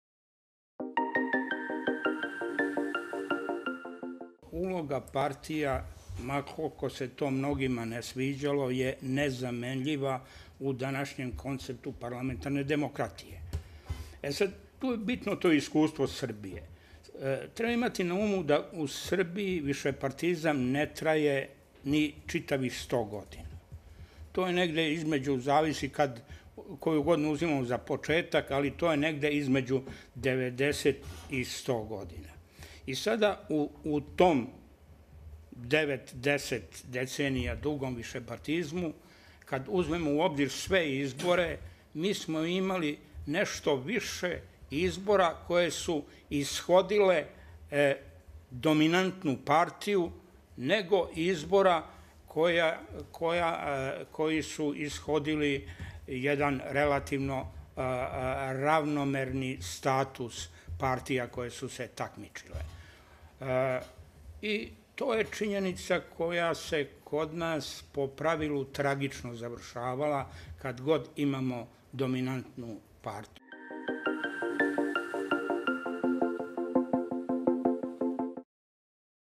Srećko Mihailović - glavni istraživač Demostata - DOMINANTNA PARTIJA
Istraživačka grupa Demostata - Naš pogled na beogradske izbore